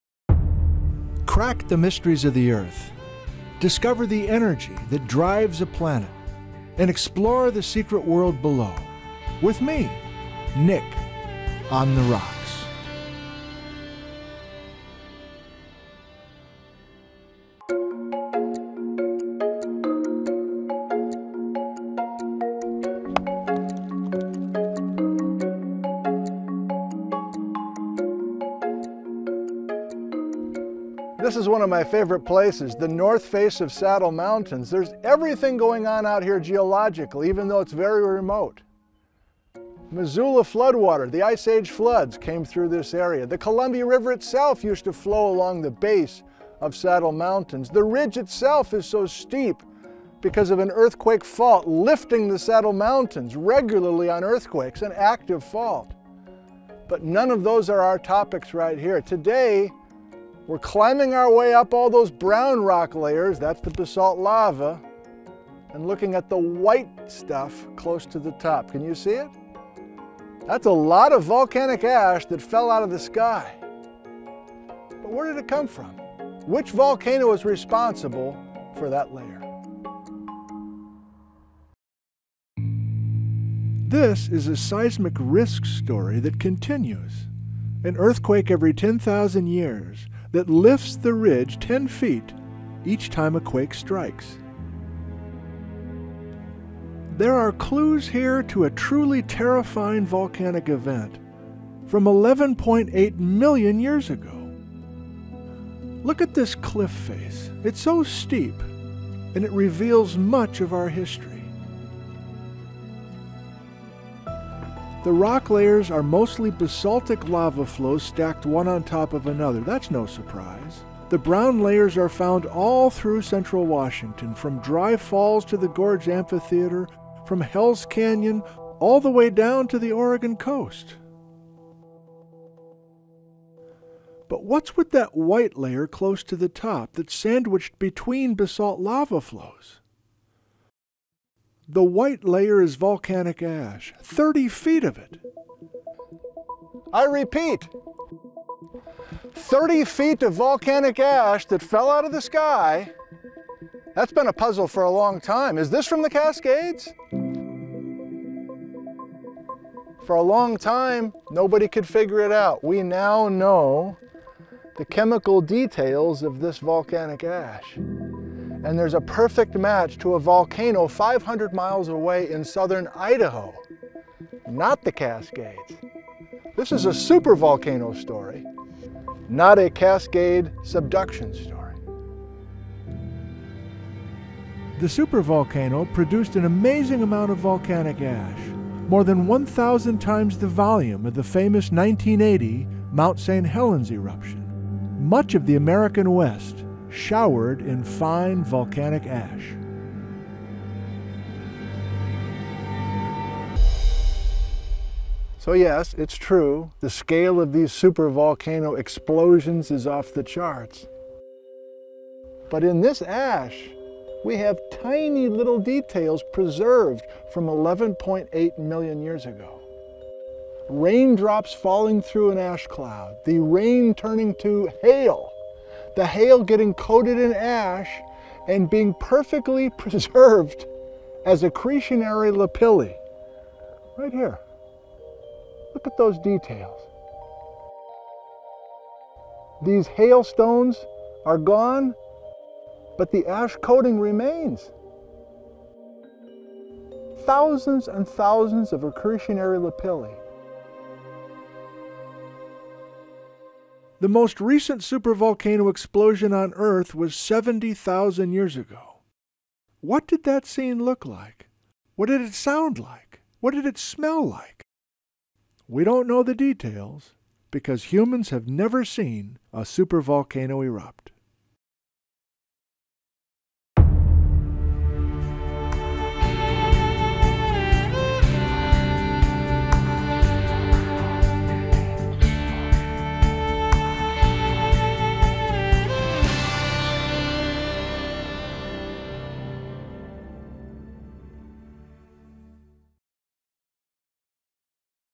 Saddle Mountains Buried in Ash | Nick on the Rocks
Thirty feet of volcanic ash in Washington’s Saddle Mountains fell out of the sky from a supervolcano explosion south of Boise, Idaho.